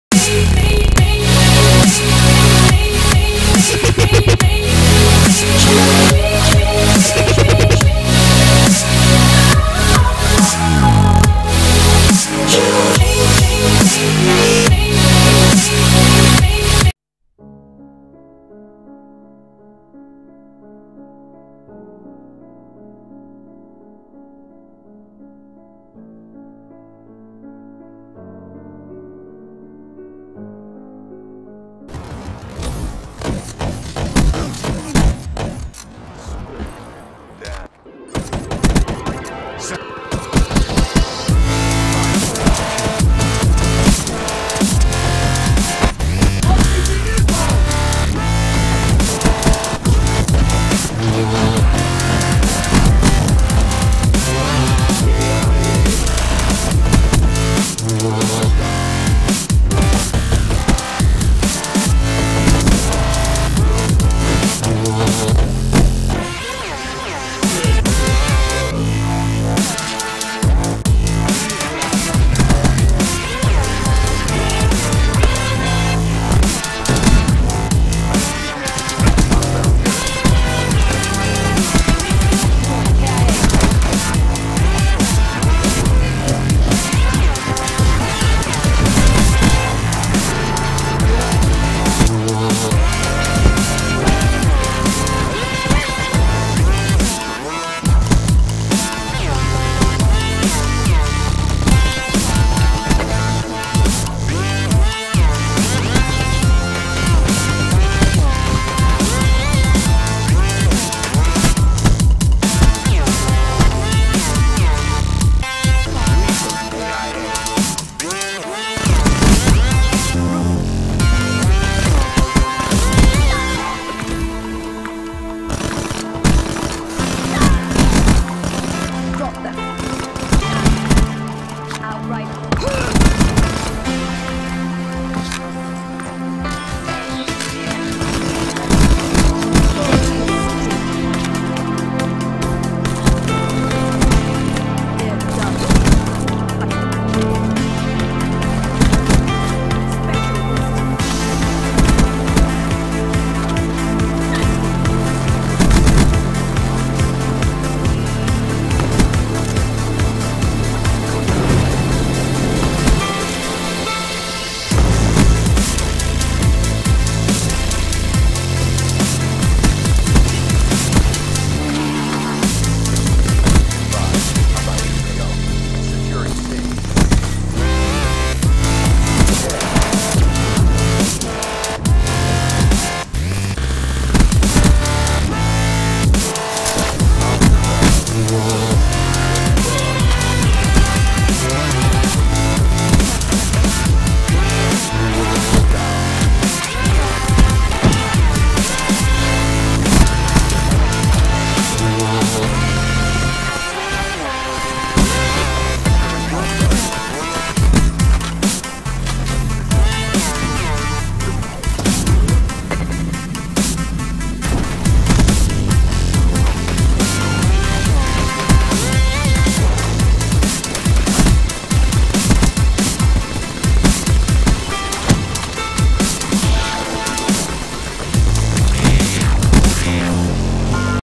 epic turn on montage
hello everyone I have for u guys a montage with some of my great clips turning on on people is the best lol enjoy and don't forget to sub for the giveaway